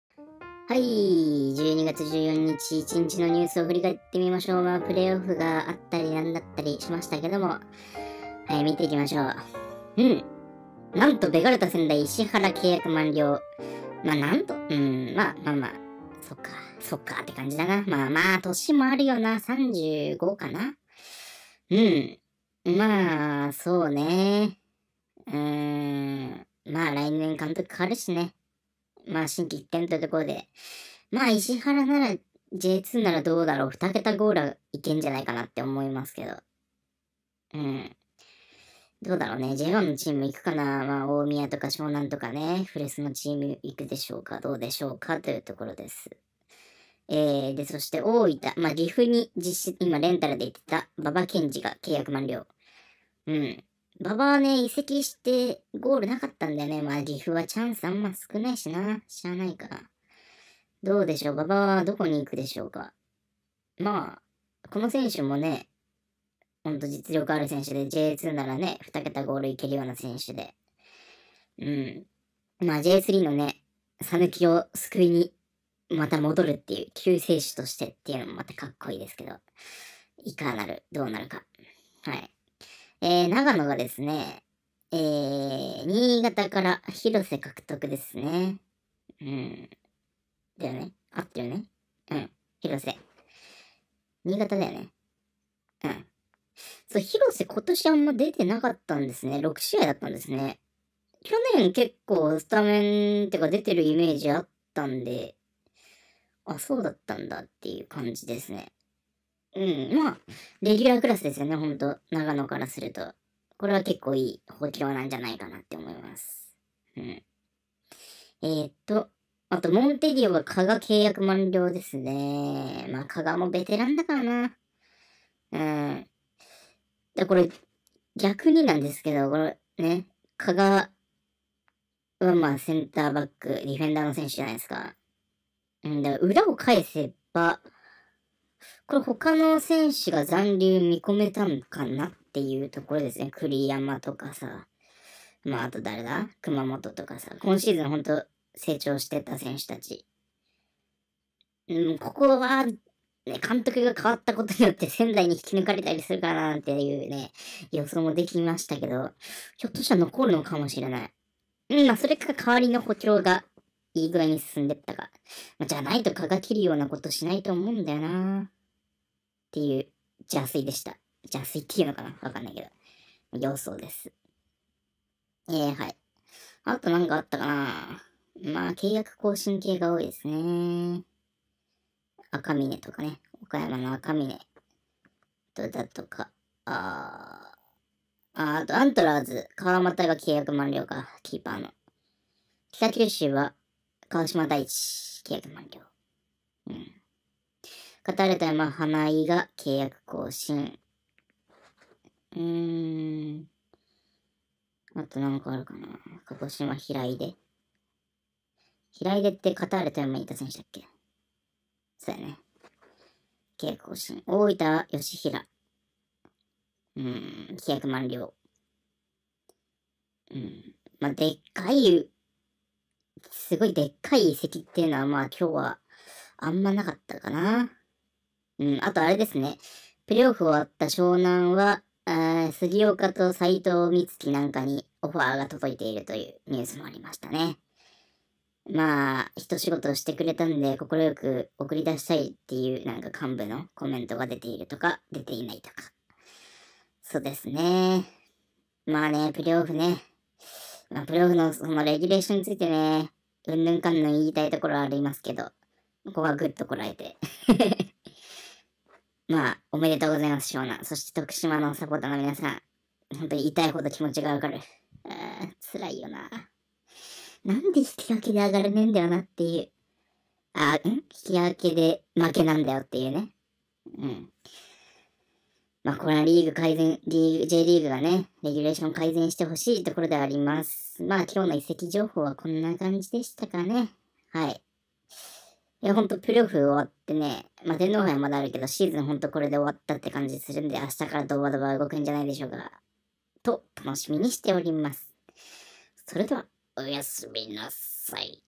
【Jリーグ移籍情報】ベガルタ仙台石原契約満了、大分トリニータ馬場契約満了、AC長野パルセイロ広瀬獲得、モンテディオ山形加賀契約満了
あんま動かなかったイメージな今日この頃です